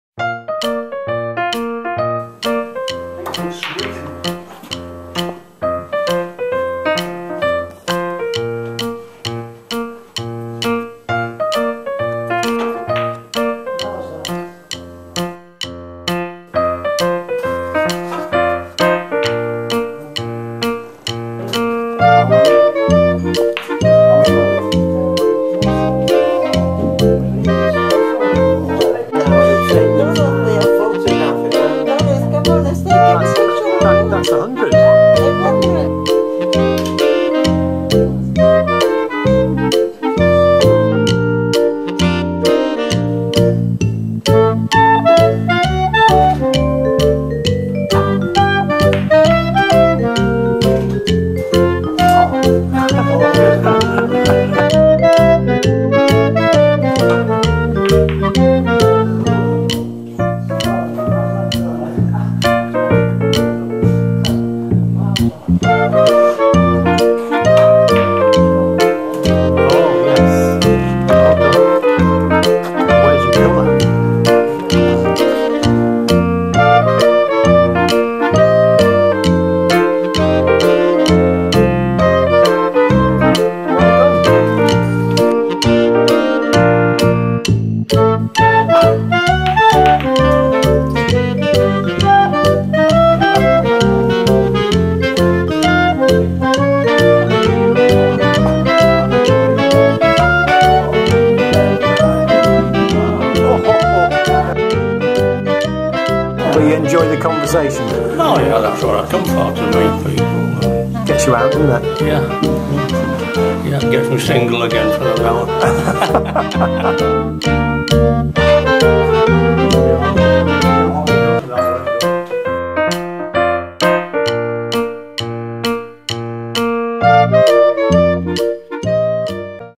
Chorlton Good Neighbours Snooker afternoons
Every Tuesday some of us go to a local cricket club just across the road from Chorlton Good Neighbours to play snooker, darts or just have a coffee and a chat. After trying out as a visitor you have to join  the club if you want to come regularly, but it is great value (a mug of tea costs 50p !)  and we always have a great time there